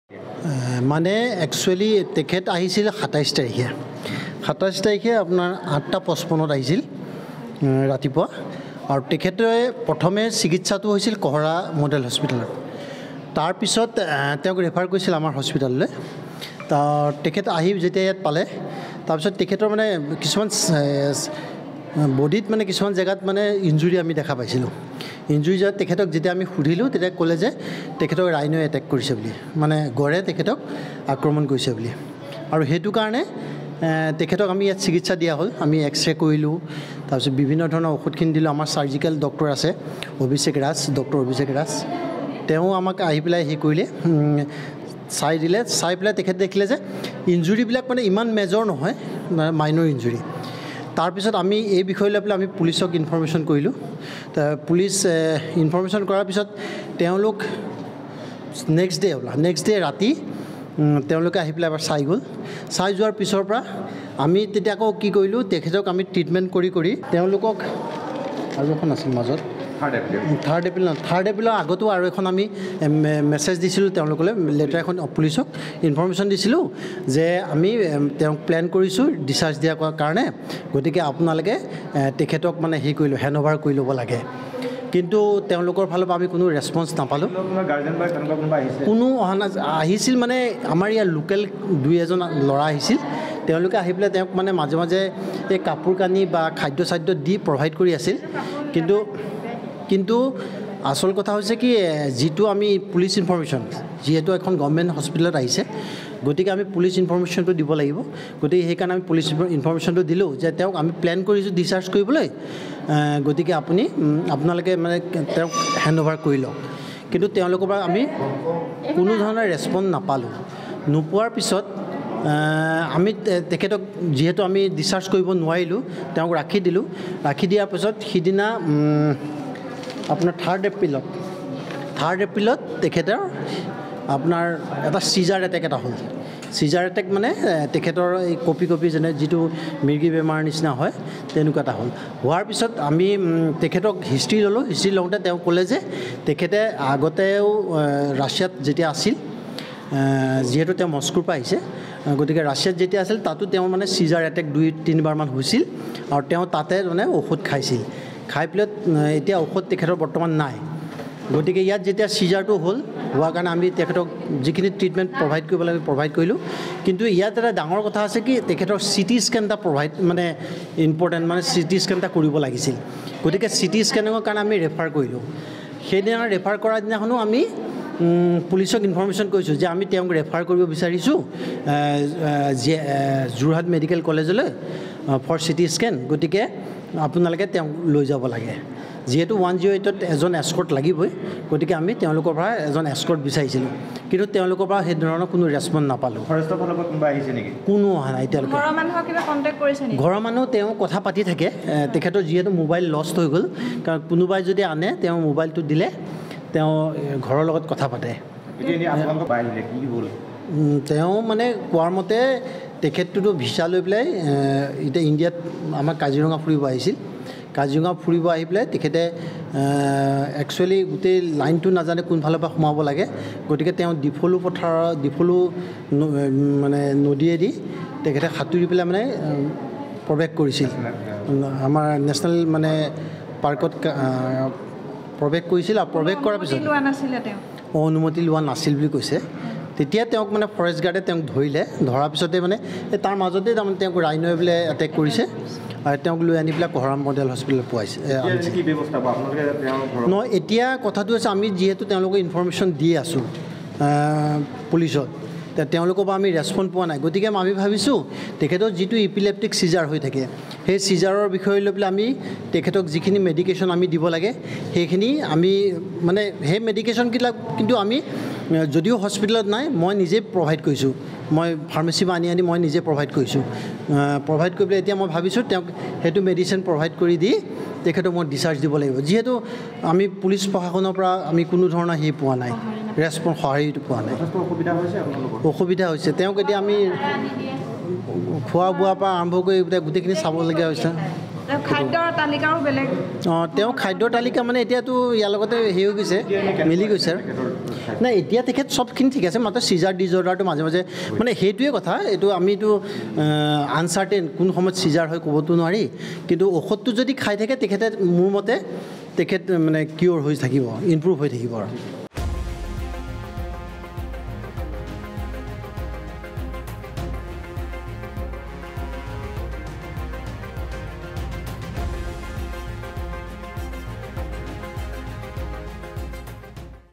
সুস্থ হৈ উঠিল কাজিৰঙাত গঁড়ৰ আক্ৰমণত আহত ৰাছিয়াৰ পৰ্যটকজন
▶️ সুস্থ হৈ উঠিল কাজিৰঙাত গঁড়ৰ আক্ৰমণত আহত ৰাছিয়াৰ পৰ্যটকজন। শ্বহীদ কমলা মিৰি অসামৰিক চিকিৎসালয়ত ১০ দিনে চিকিৎসাধীন হৈ আছিল ৰাছিয়ান পৰ্যটক নিকিতা কৰিয়াভস্কিন।